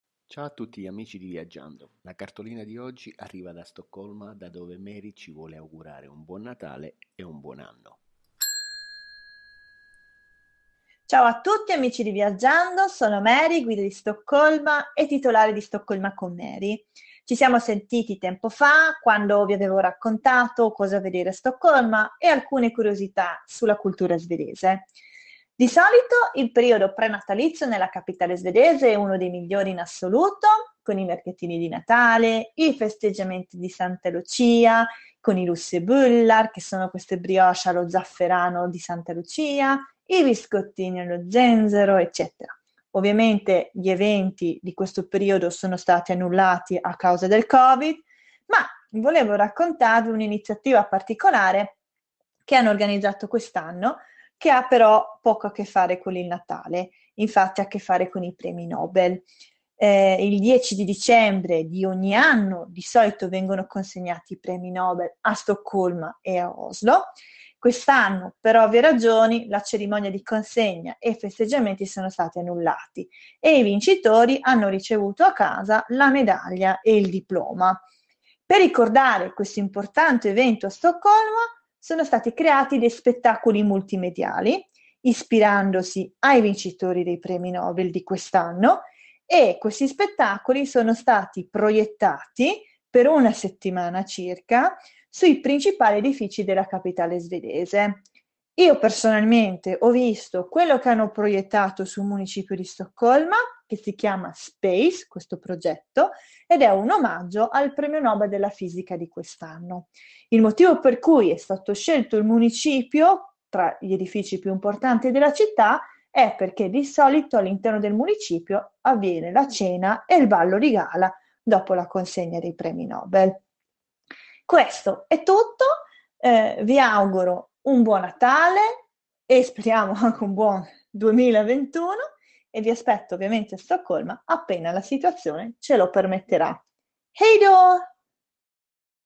Cartolina di Natale da Mary da Stoccolma
Nella puntata di oggi Mary ci augura un Buon Natale da Stoccolma